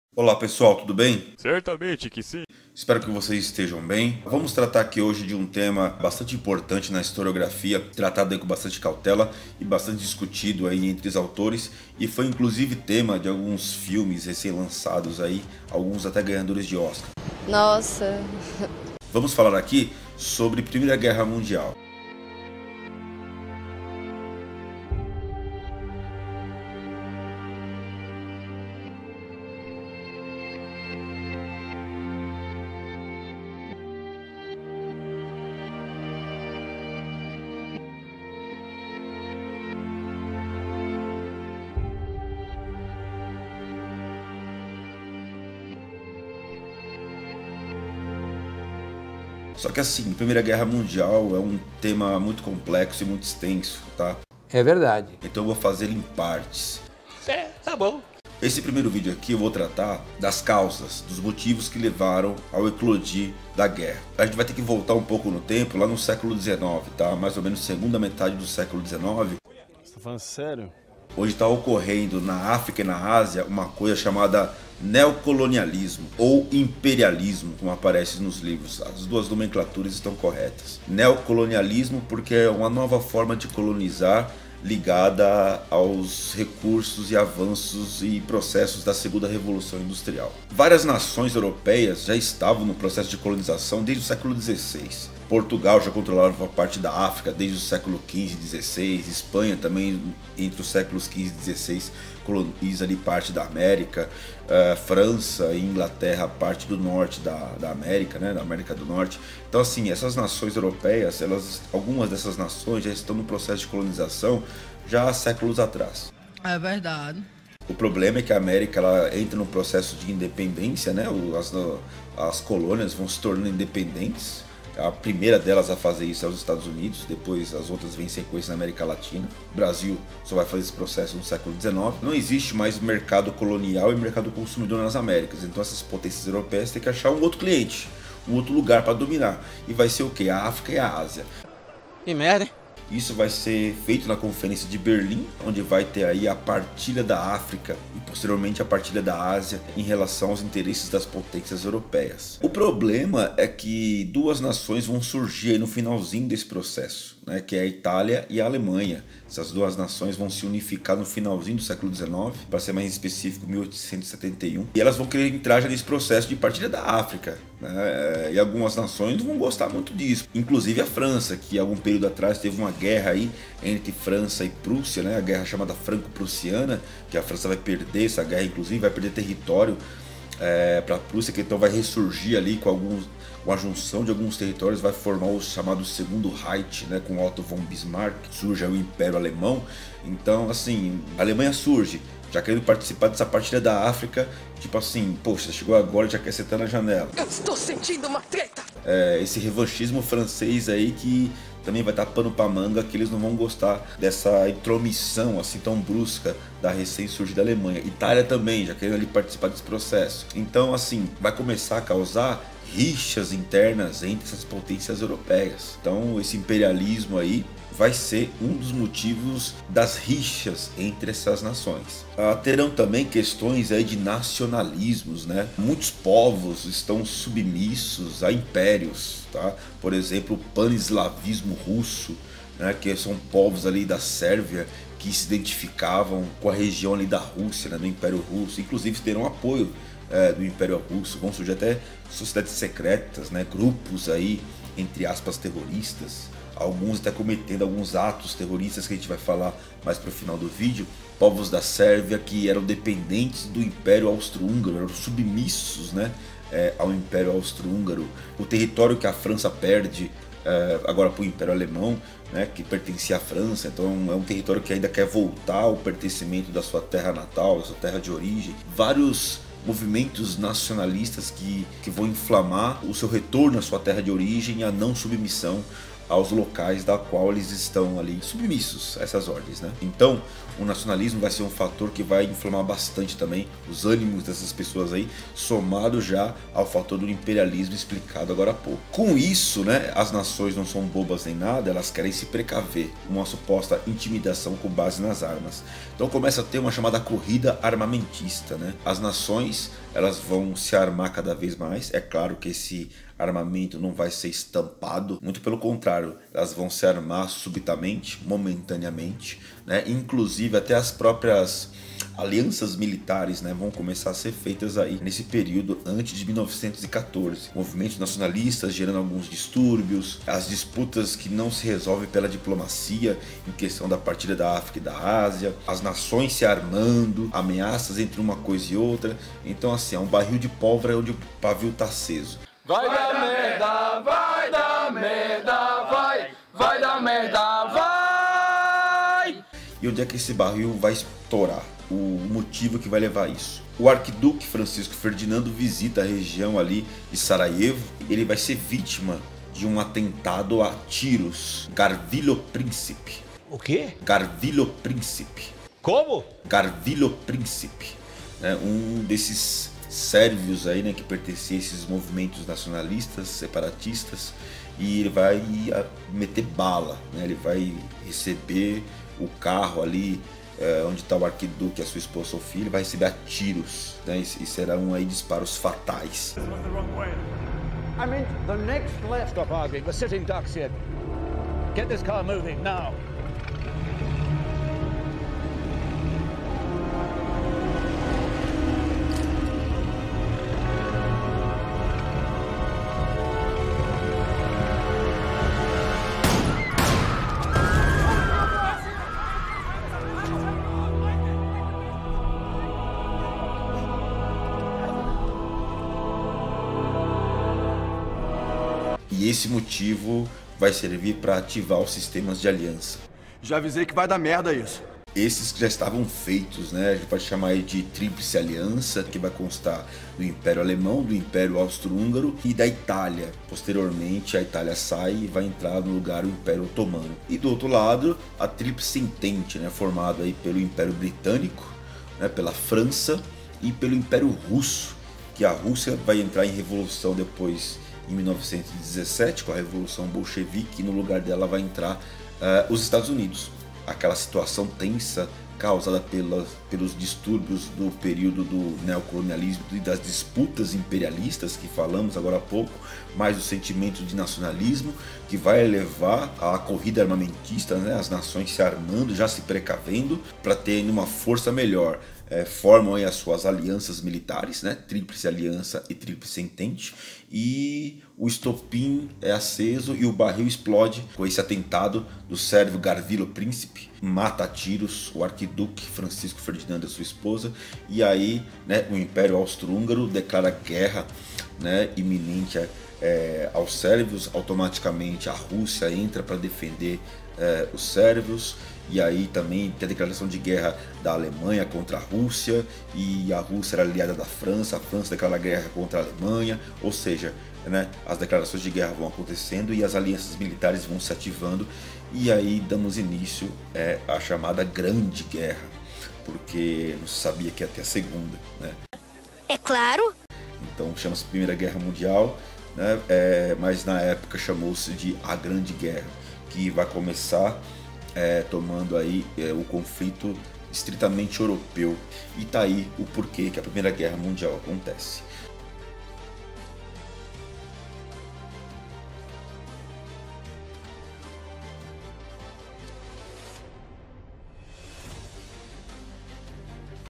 A Grande Guerra - Você sabe como começou a 1º Guerra Mundial? (PARTE 1)
Neste vídeo iremos fazer alguns apontamentos sobre o que levou ao criado de um dos maiores conflitos da história da humanidade, a Primeira Guerra Mundial (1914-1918)